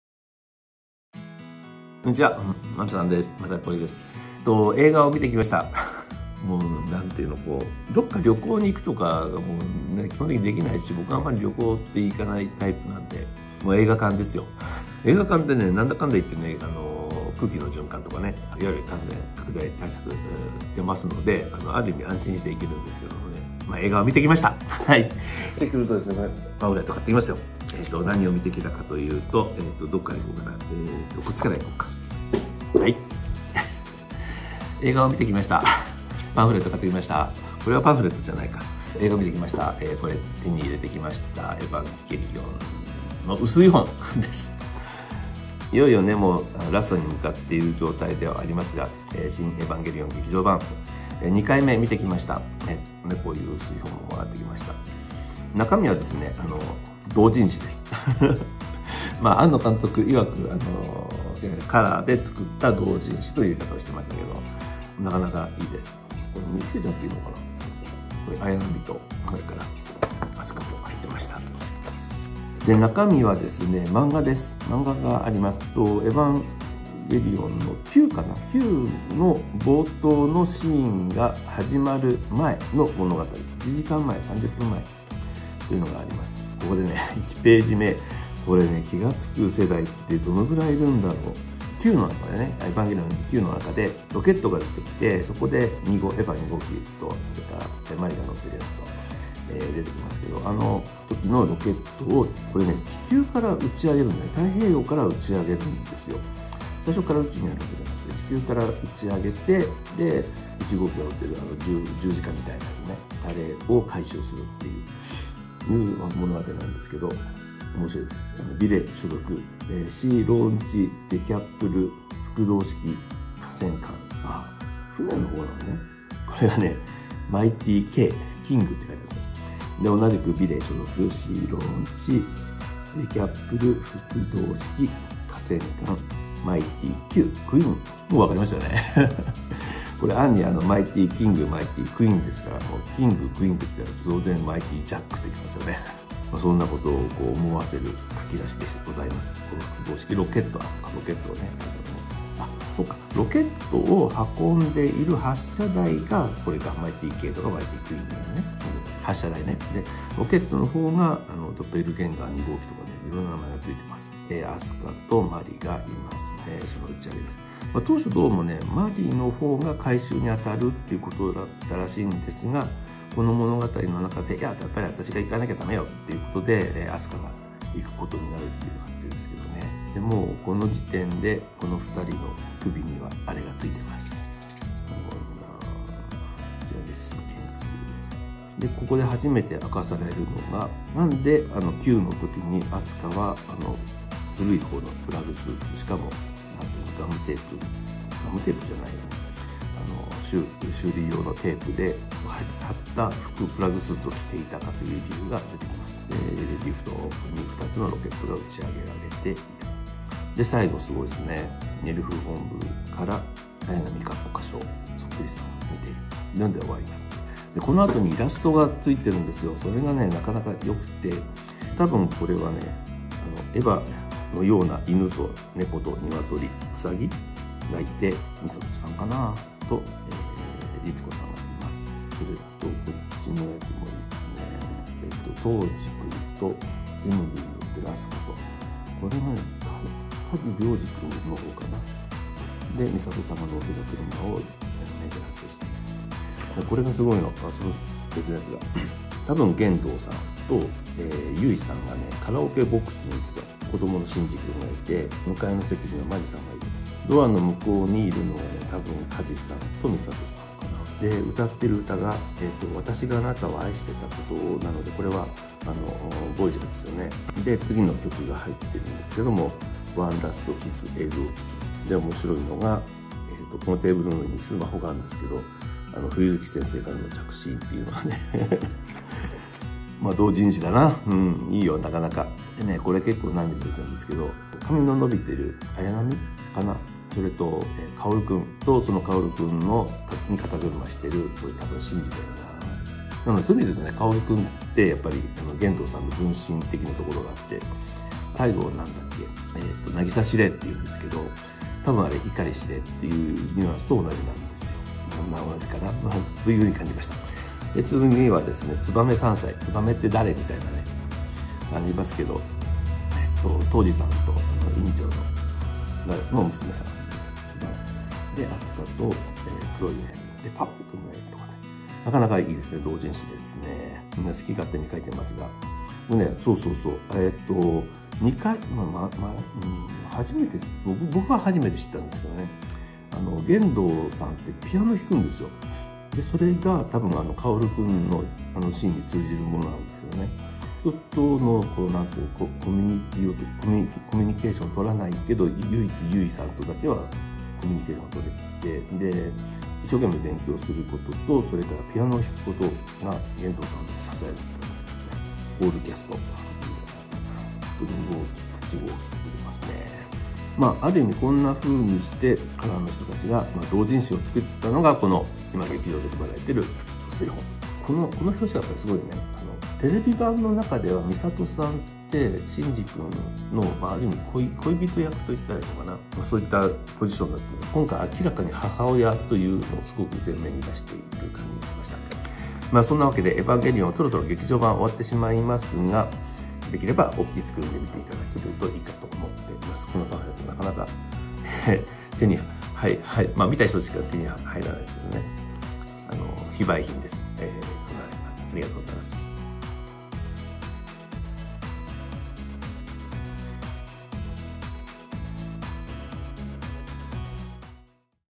シン・エヴァンゲリオン劇場版 EVA EXTRA EXTRA 薄い本（36ページ）をめくりながら - まる３のパンフレットレビュー
こんにちは、まる３です。

映画『シン・エヴァンゲリオン劇場版』の２回目の鑑賞で、この薄い本をいただきました。EVA EXTRA EXTRA です。
株式会社カラーがつくった公式の同人誌（薄い本）です。

薄い本の冒頭は、映画『ヱヴァンゲリヲン新劇場版:Q』の最初のシーンの1時間くらい前の物語。当初はマリがシンジを吸収したエヴァ初号機を回収する予定だったのが、アスカが回収を決心するくだりが描かれています。ネーミングの小物たちとして、マイティーＫ（キング）、マイティーＱ（クィーン）ってあるのは、その先にたぶんもうひとつ マイティーＪ（ジャック）を含んでいるのかな？漫画の最後のカットには、その後登場する黒いアヤナミレイ（仮称）の姿も。

漫画のあとは色々な方々のイラストが掲載されていますが、一枚一枚にこめられた想いを感じながら、ページをめくるのがおすすめです。